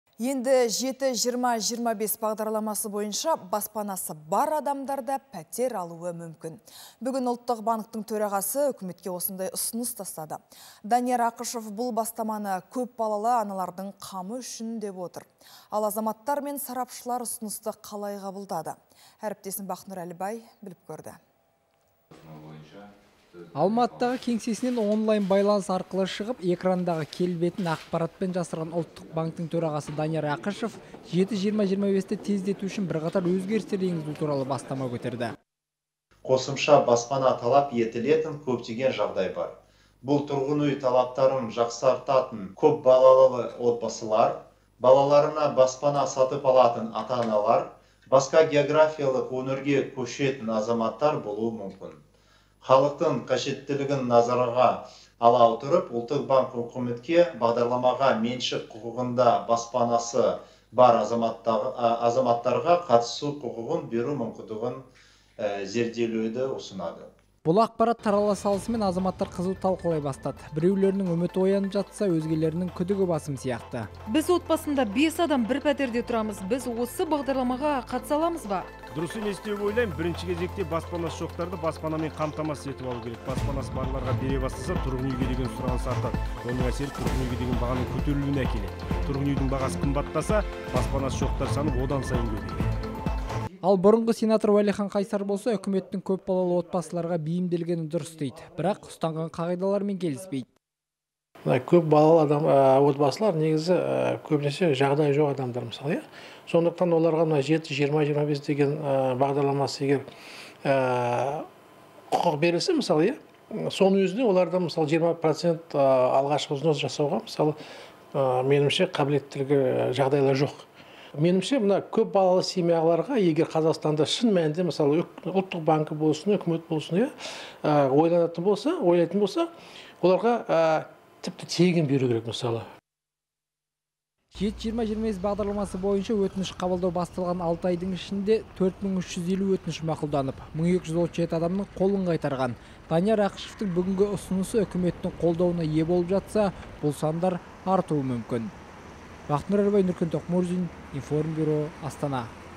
«7-20-25»-пен үй алудың шарттары өзгерді
Енді «7-20-25» бағдарламасы бойынша баспанасы бар адамдар да пәтер алуы мүмкін. Бүгін Ұлттық банктің төрағасы үкіметке осындай ұсыныс тастады. Данияр Ақышев бұл бастаманы көп балалы аналардың қамы үшін деп отыр. Ал, азаматтар мен сарапшылар ұсынысты қалай қабылдады?